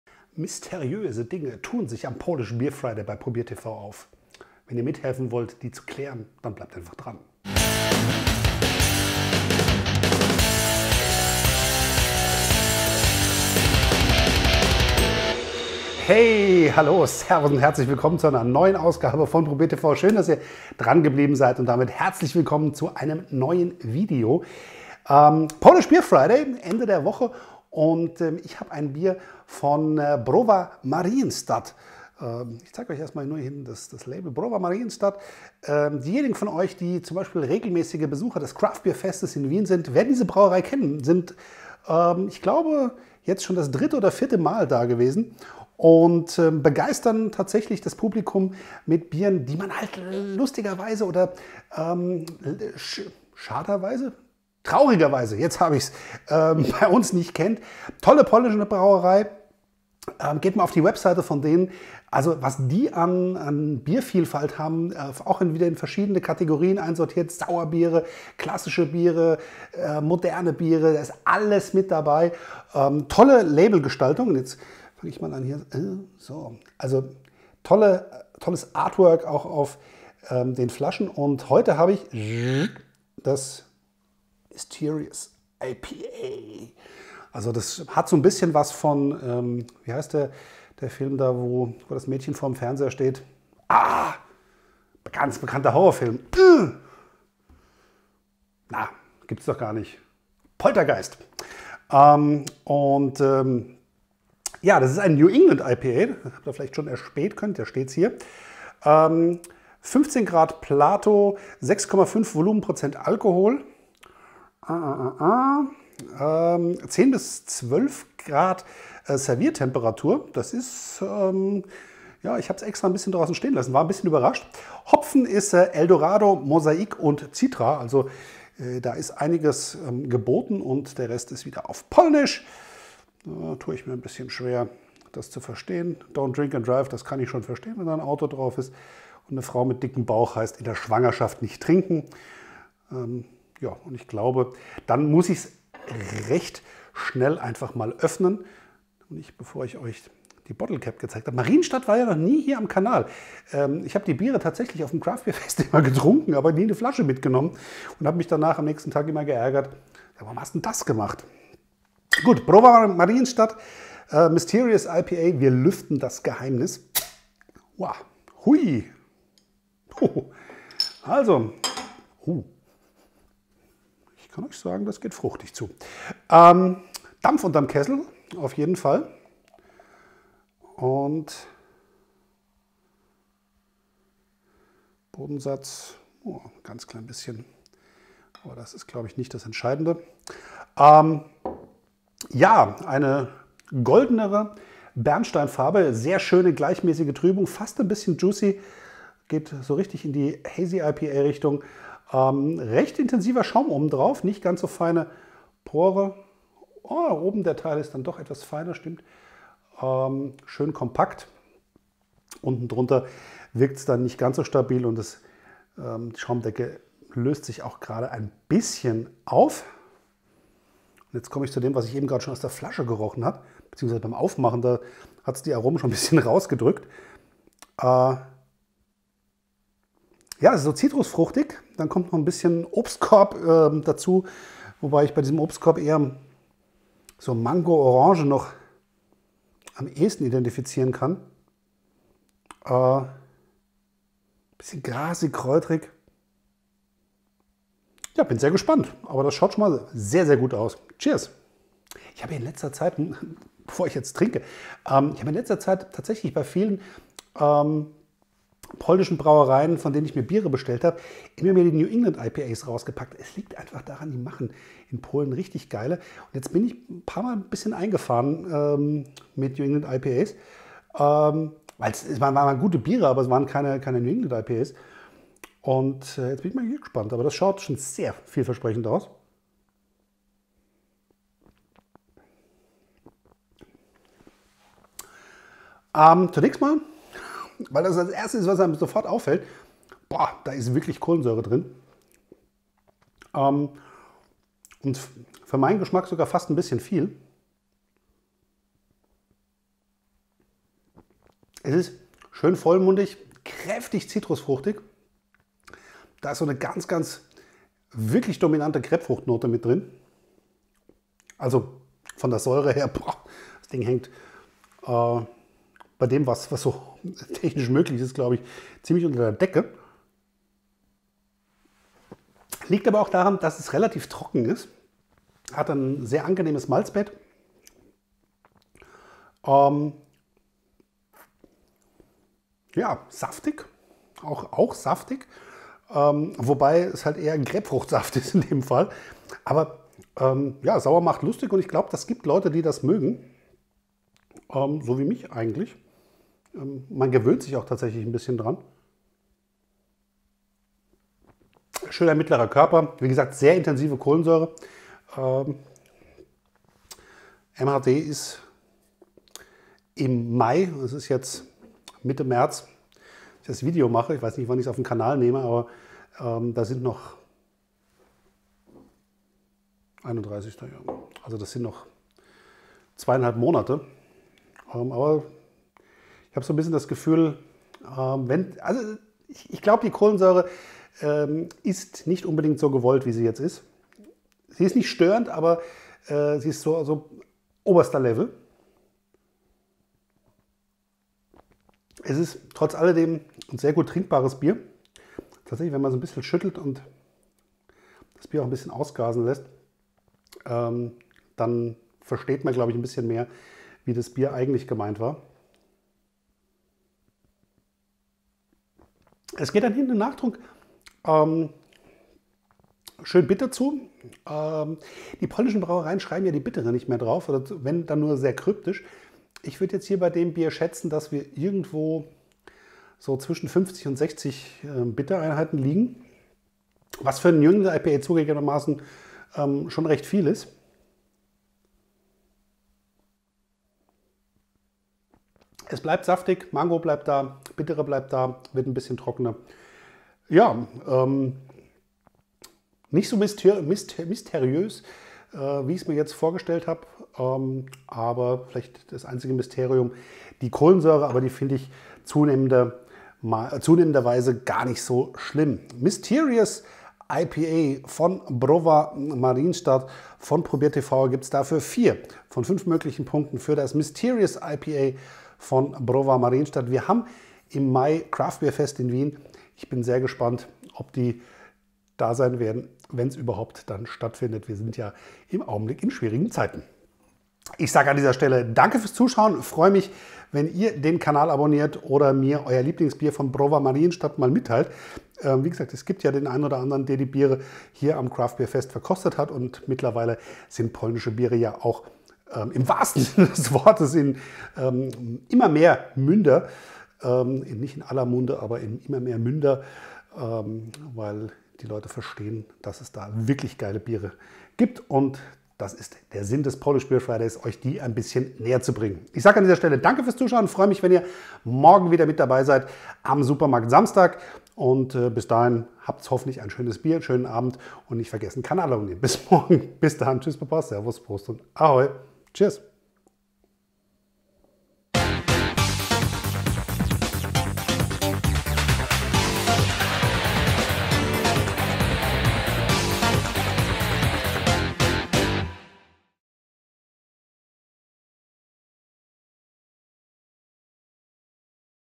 Mysterious IPA von Maryensztadt | proBIER.TV - Craft Beer Review #1249 [4K]
Mysterious IPA von Maryensztadt, Polen

India Pale Ale, Polishbeerfriday, Wroclaw, Craftbeer, Craftbier